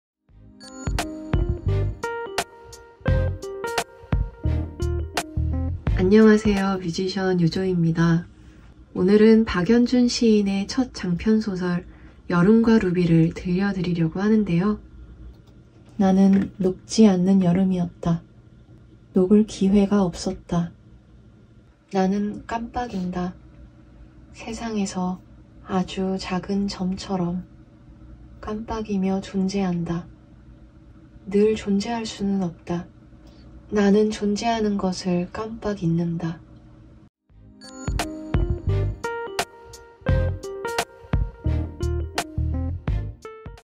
뮤지션 요조가 읽는 박연준 소설 『여름과 루비』 오디오북 사전 공개
박연준 시인 장편소설 『여름과 루비』 오디오북 녹음에 뮤지션 요조 님께서 참여해주셨습니다. 깊고 섬세한 문장들을 요조 님의 목소리로 들으니 금세 빠져들 수 밖에 없었는데요.

녹음이 끝난 후 요조 님은 "1시간 반 동안 녹음하는 내내 행복했다. 글로 읽었을 때도 좋았지만 말로 내뱉으니 더 좋았다. 녹음하는 사람도 듣는 사람도 행복한 순간이었다."라고 말씀해주셨습니다.

현재 열심히 제작 중!
🎧 요조가 읽는 『여름과 루비』 오디오북, 곧 만나요!